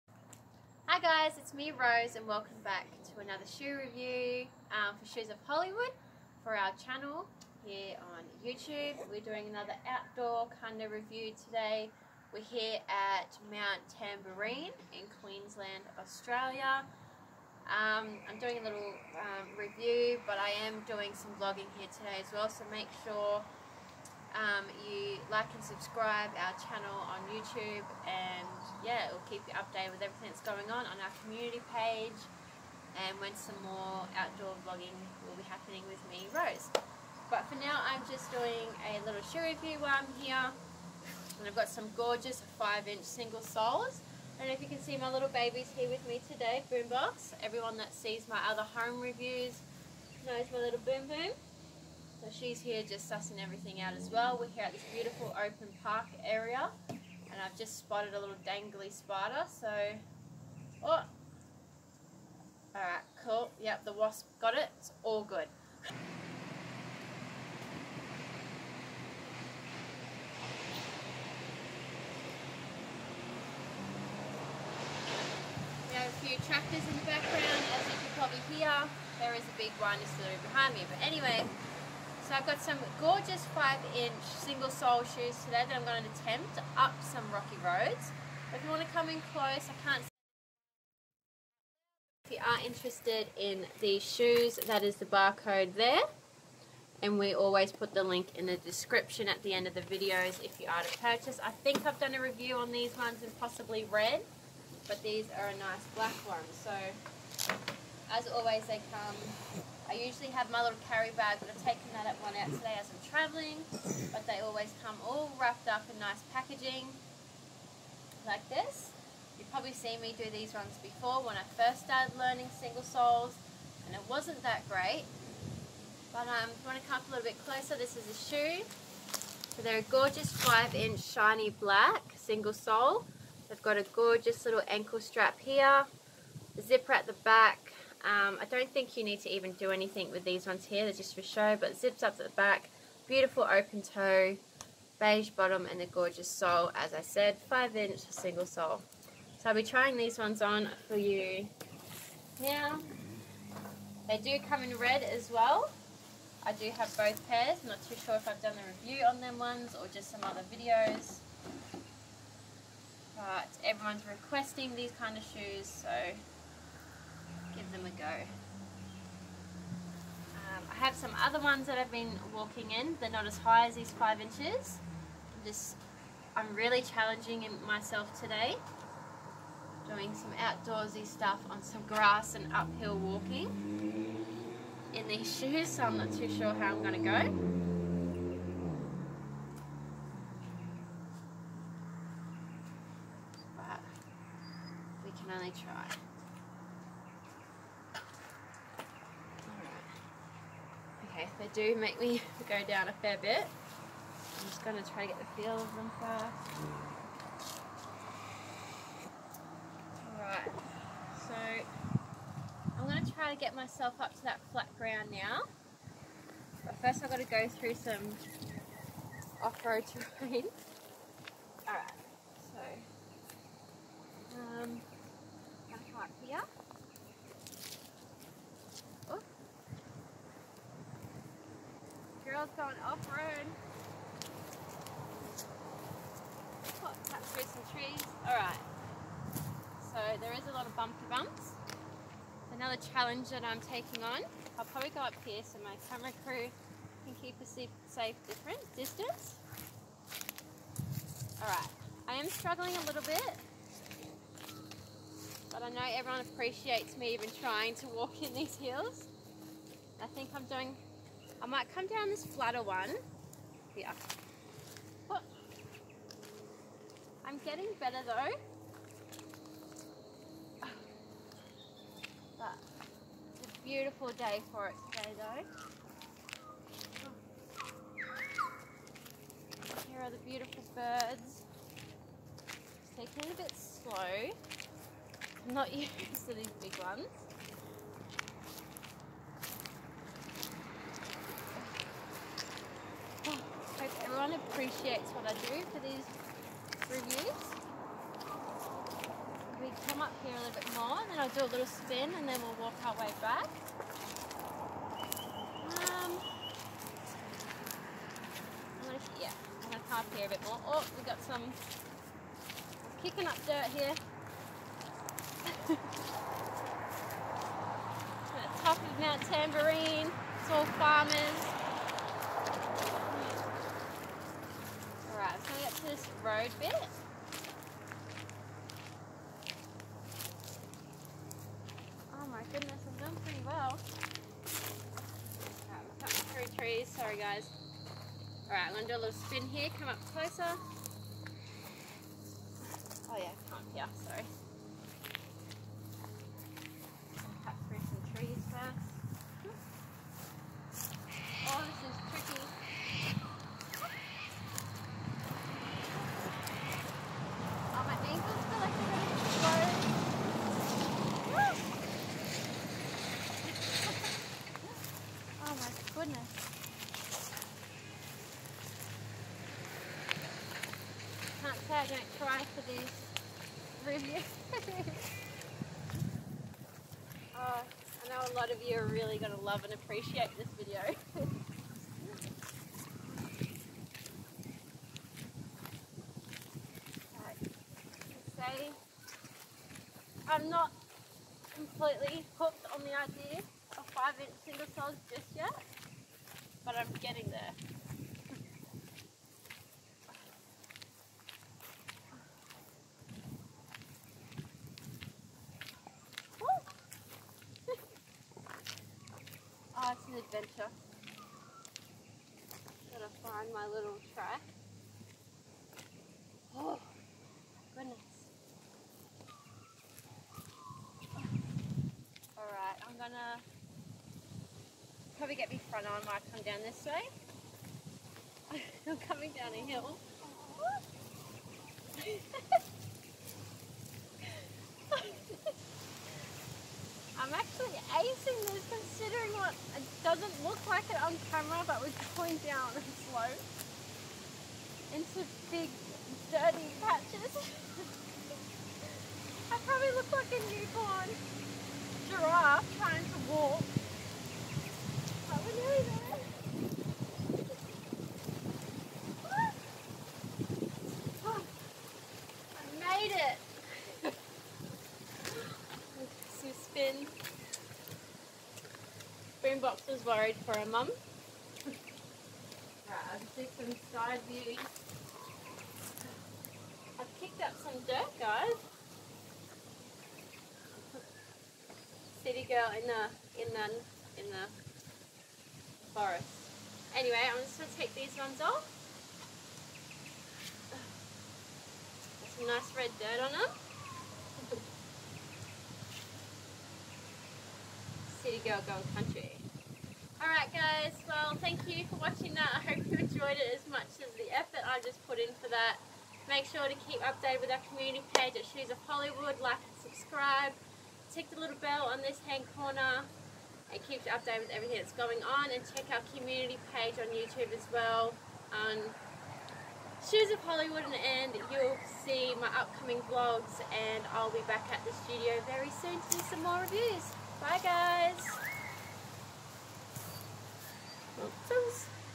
Rose Takes Some 5 Inch Pleaser Single Sole High Heel Shoes for a Hill Climb Walk and In The Grass
Rose is at Mount Tambourine in Queensland Australia and Tries out some Pleaser 5 Inch Single Sole High Heel Shoes and goes for a Hill Climb Walk to Test Them Out in more Tricky Conditions than the usual Car Park Walk.